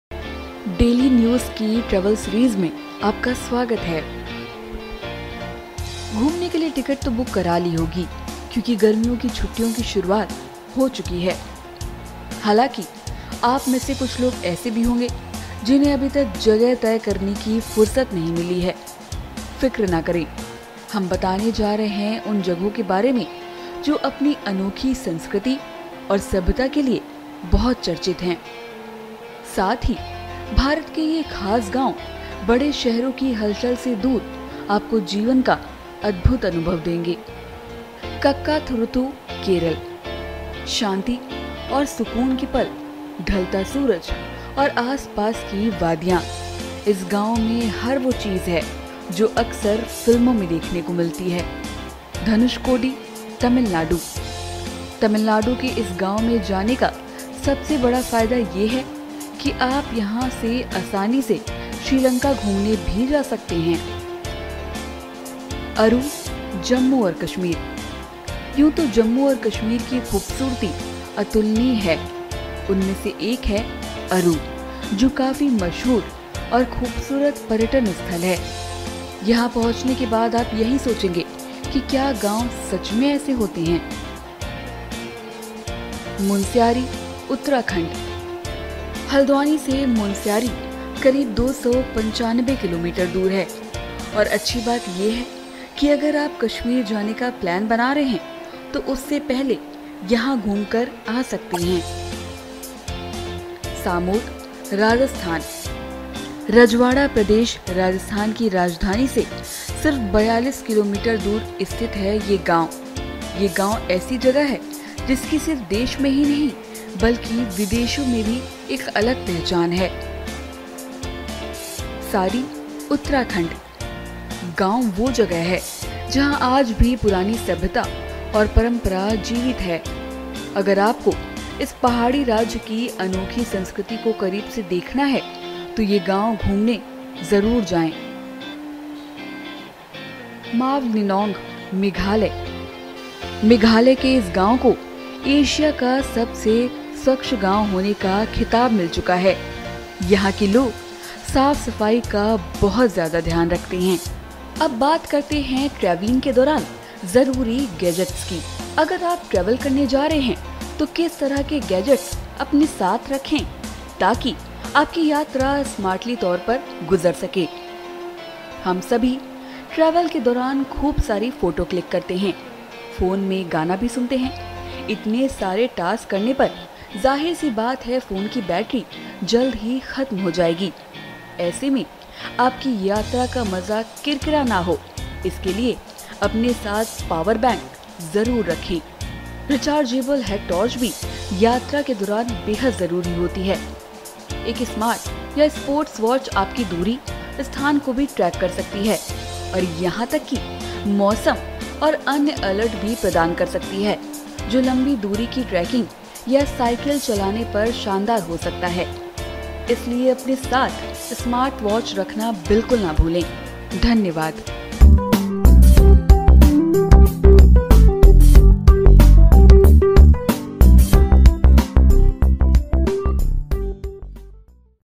ये जगहें आपको देंगी जीवन का अद्भुत अनुभव, सफर का उठा सकेंगे भरपूर लुत्फ
Places wonderfulexperience life fun Kakkathuruthu Dhanushkodi Aru JammuandKashmir munsiyari Kakkathuruthutourism Dhanushkoditourism Arutourism JammuandKashmirtourism munsiyaritourism घूमने के लिए टिकट तो बुक करा ली होगी क्योंकि गर्मियों की छुट्टीयों की शुरुआत हो चुकी है। हालांकि आप में से कुछ लोग ऐसे भी होंगे जिन्हें अभी तक जगह तय करने की भी फुर्सत नहीं मिली है। फिक्र ना करें हम बताने जा रहे उन जगहों के बारे में जो अपनी अनोखी संस्कृति और सभ्यता के लिए चर्चित है। साथ ही भारत के ये खास गांव बड़े शहरों की हलचल से दूर आपको जीवन का अद्भुत अनुभव देंगे।